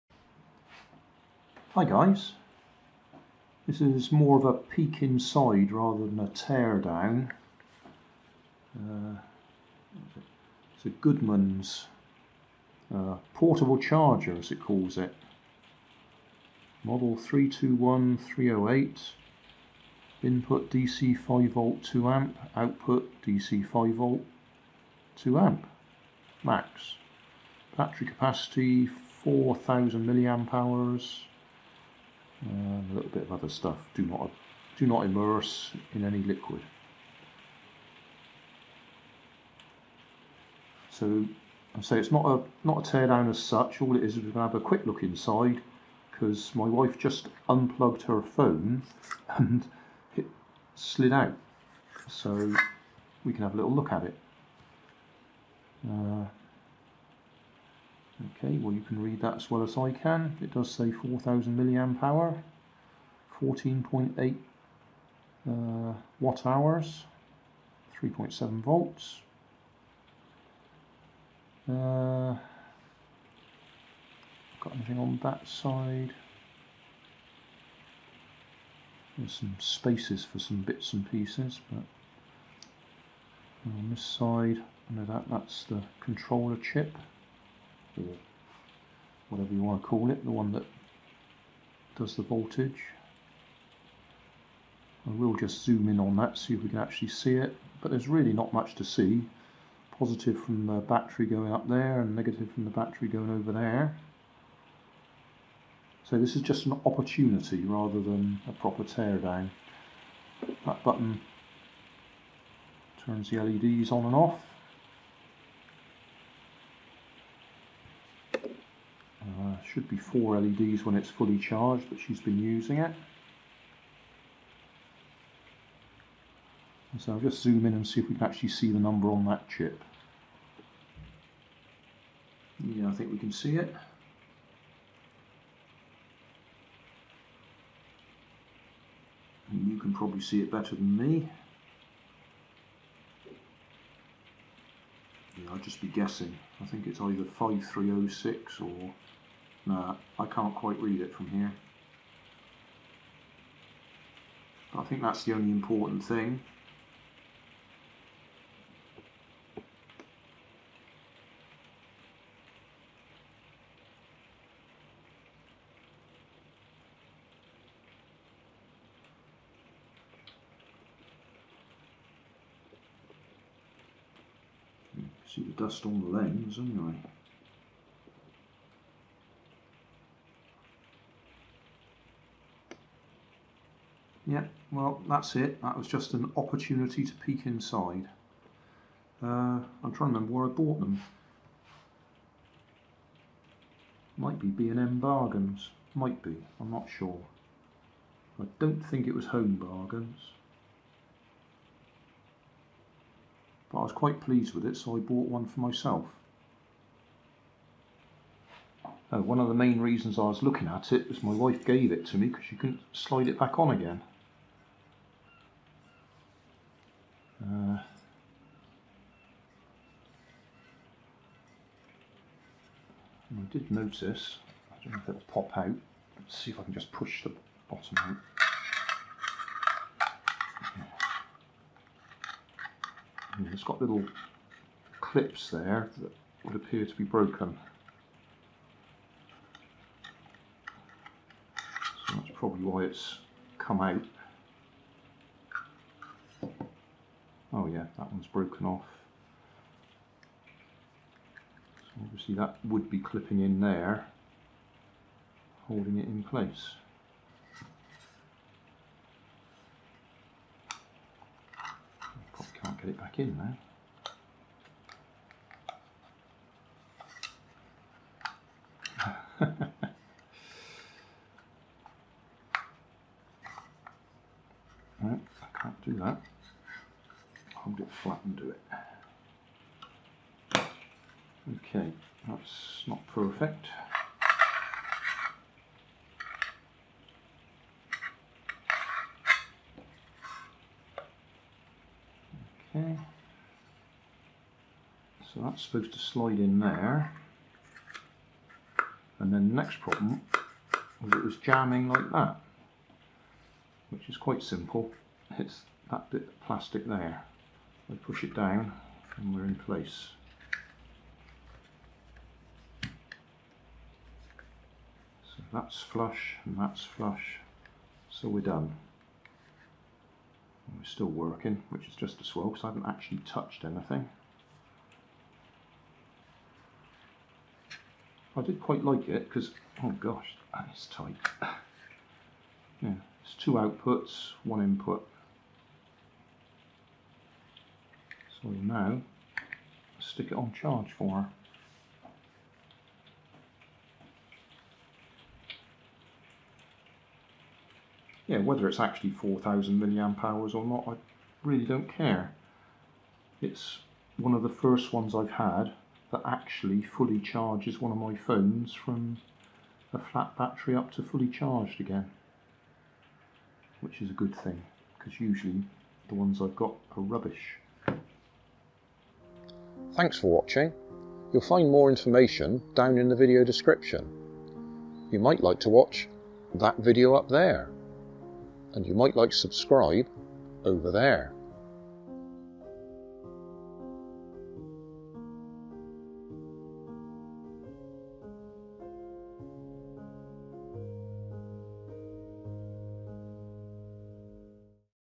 Peek inside Goodmans portable charger 4000mAh
Peek inside Goodmans portable charger 4000mAh. I picked this up for £6.99 in B&M Bargains, works well enough for my purposes, it actually fully charges my wifes phone from flat, which most of these cheap power banks fail to do. I was so impress I went back and bought one for myself. Today my wife showed me the entire assembly came out when she tried to unplug her phone. Annoying but not the end of the world and it gave me a chance to have a peek inside.

I found this information on that chip
The IP5306 is a multi-function power management SOC with an integrated boost converter, lithium battery charge management, and battery power indication, providing a complete power solution for mobile power supplies.
The high integration and rich features of the IP5306 make it possible to use only a few external components in the application and to effectively reduce the overall solution size and BOM cost.

Features
1. Synchronous switch charge and discharge
2. 2.4A Synchronous Boost Converter, 2.1A Synchronous Switch Charge
3. Boost efficiency up to 92%
4. Up to 91% charge efficiency

Applications

1. Mobile Power / Charging Po
2. Mobile phones, tablet PCs and other portable devices